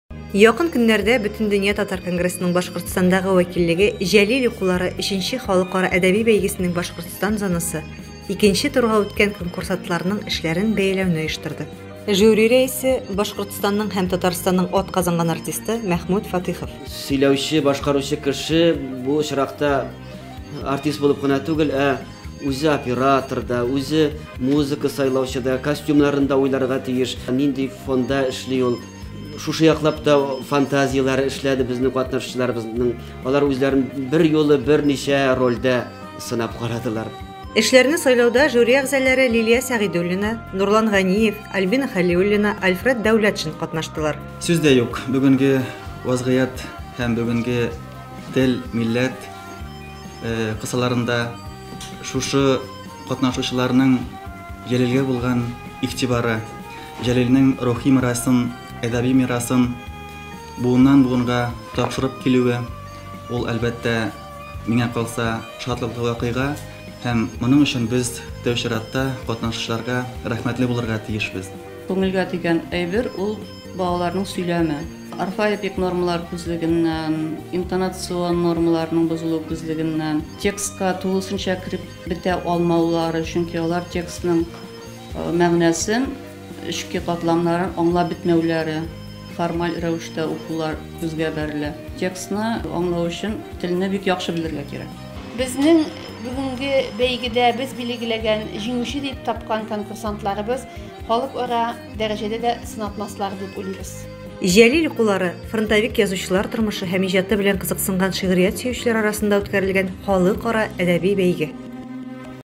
Җәлил укулары
«Җәлил укулары» III Халыкара әдәби бәйгесенең Башкортстан зонасы - 2 турга үткән конкурсантларның эшләрен бәяләү мизгелләреннән видеоязма карарга тәкъдим итәбез!